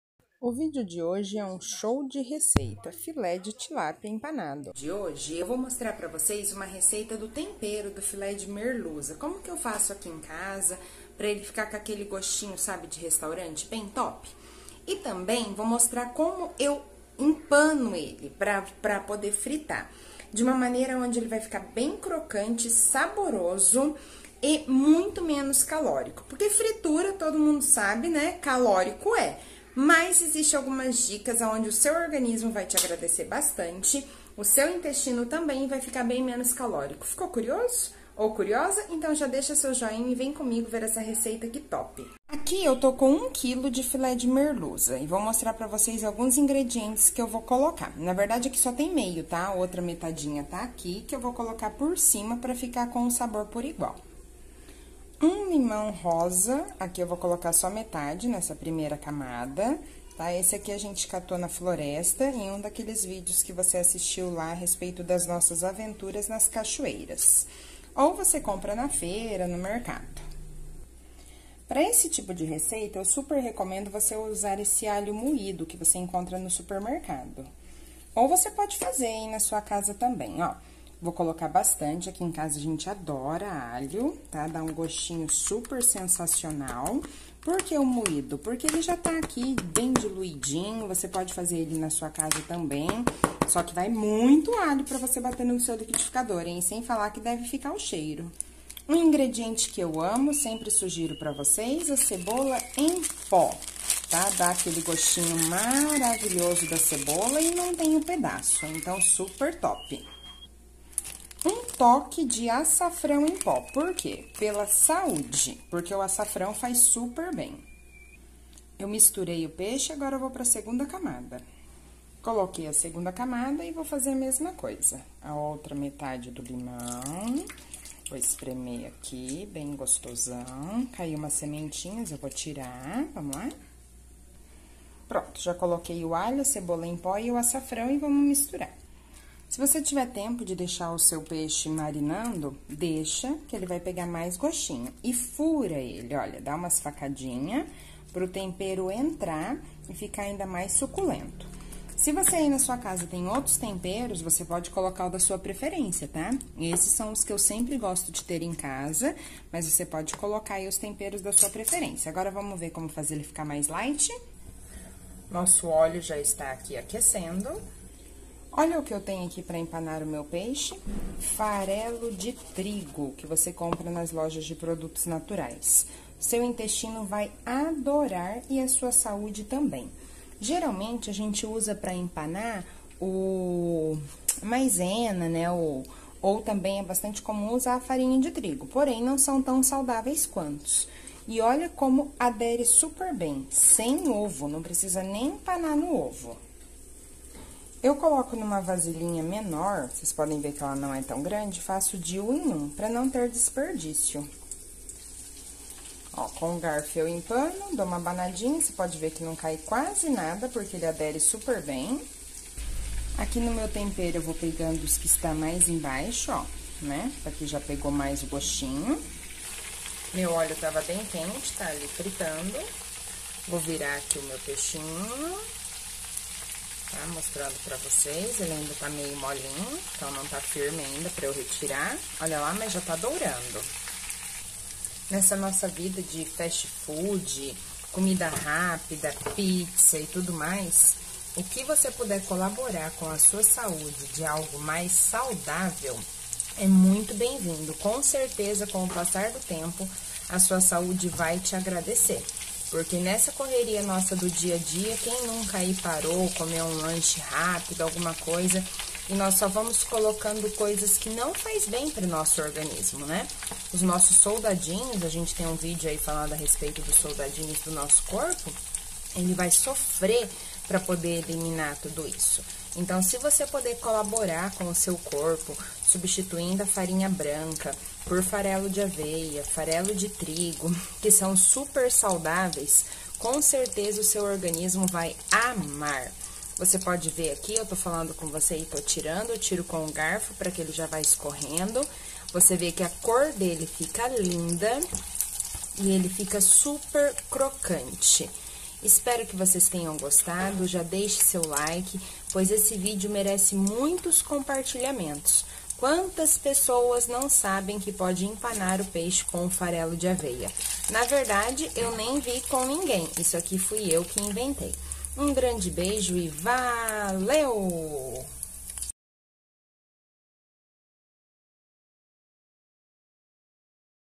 FILÉ DE TILÁPIA EMPANADO COM FARELO DE TRIGO. RECEITA DELICIOSA E FIT.
Ajuda muito nós e é um super incentivo para mais receitinhas no canal.
Rumo aos 100 mil!

VOCÊ ADORA FILÉ DE TILÁPIA FRITINHO CROCANTE COM AQUELE GOSTINHO DE RESTAURANTE
PORÉM SE PREOCUPA DEVIDO A FRITURA SER SEMPRE MUITO CALÓRICO
POIS ESSE VÍDEO FOI FEITO ESPECIALMENTE PARA VOCÊ
AQUI EU VOU LHE ENSINAR PASSO A PASSO SOBRE O TEMPERO DO FILÉ DE TILÁPIA E COMO VOCÊ PODE EMPANA LO COM FARELO DE TRIGO FAZENDO COM QUE FIQUEM MUITO MAIS SAUDÁVEL CROCANTE E SABOROSO
SEU INTESTINO E A SUA SAÚDE AGRADECE ESPERO QUE VOCÊ GOSTE